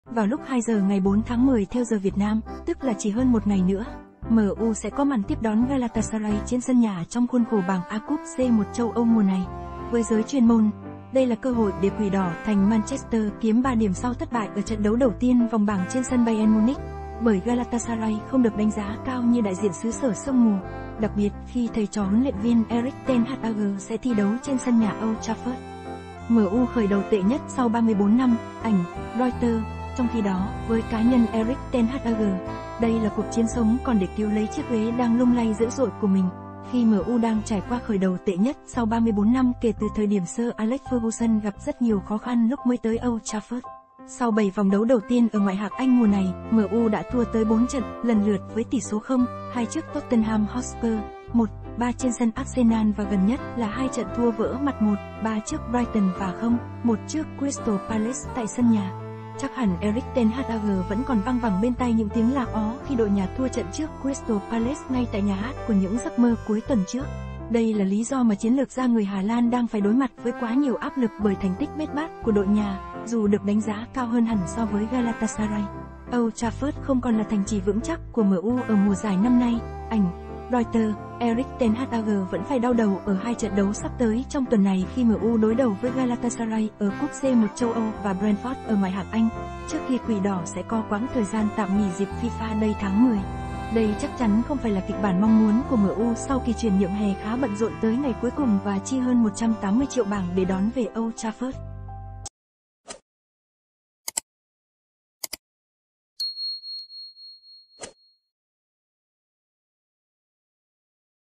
MU ĐỐI ĐẦU GALATASARAY | CƠ HỘI THẮNG ĐỂ XOAY CHUYỂN KHÓ KHĂN
Vào lúc 2h00 ngày 4/10 theo giờ Việt Nam, tức là chỉ hơn một ngày nữa, MU sẽ có màn tiếp đón Galatasaray trên sân nhà trong khuôn khổ bảng A Cúp C1 châu Âu mùa này. Với giới chuyên môn, đây là cơ hội để Quỷ đỏ thành Manchester kiếm 3 điểm sau thất bại ở trận đấu đầu tiên vòng bảng trên sân Bayern Munich, bởi Galatasaray không được đánh giá cao như đại diện xứ sở sương mù, đặc biệt khi thầy trò  Huấn luyện viên Erik ten Hag sẽ thi đấu trên sân nhà Old Trafford.